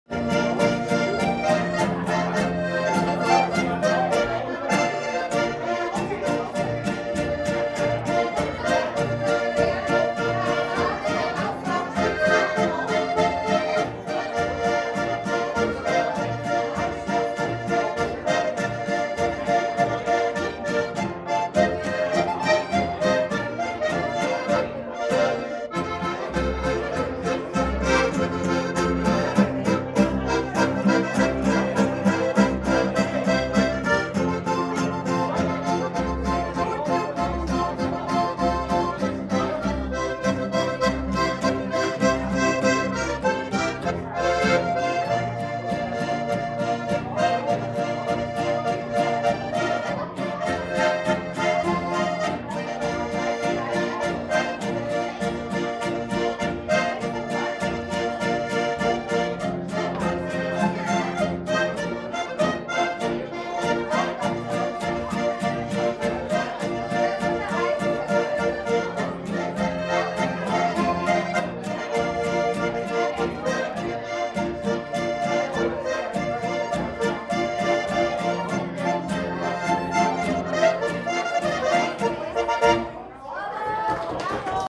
Zum Heurigen 19.04.2016-006 © OlmHERZ
flott aufgespielt "zum Heurigen" 8570 Voitsberg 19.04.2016_006 © OlmHERZ
Jeden Dienstag ab 17:00